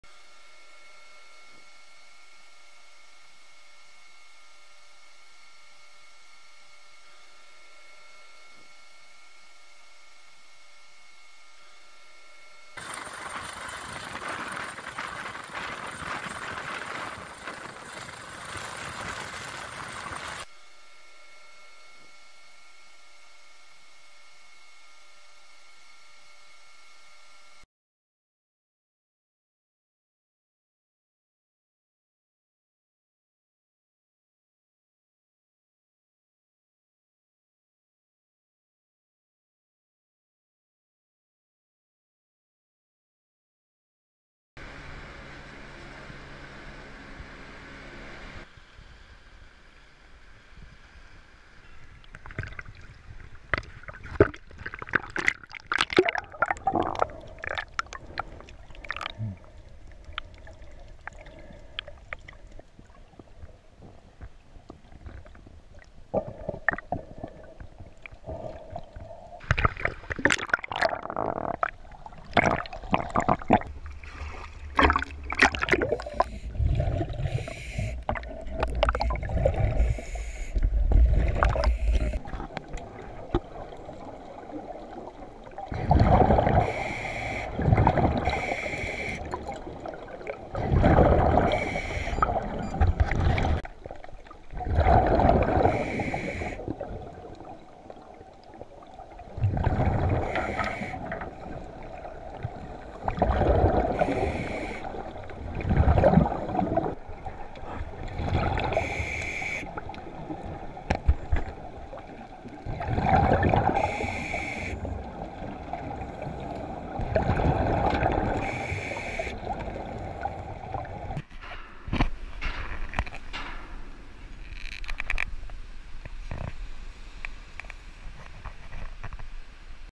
motonave CDRY Blue disastro ambientale marino, inquinamento ambient atmosferico e naufragio colposo
La Procura della Repubblica presso il Tribunale di Cagliari ha disposto nei confronti di tre persone fisiche e due persone giuridiche la notificazione dell’avviso di conclusione delle indagini preliminari relative all’incaglio e naufragio della Motonave CDRY Blue, avvenuto la sera del 21 dicembre 2019 in località Capo Sperone dell’Isola di Sant’Antioco, nel quale tutto l’equipaggio composto da 12 persone fu salvato mediante un complesso intervento degli elicotteri della Guardia Costiera,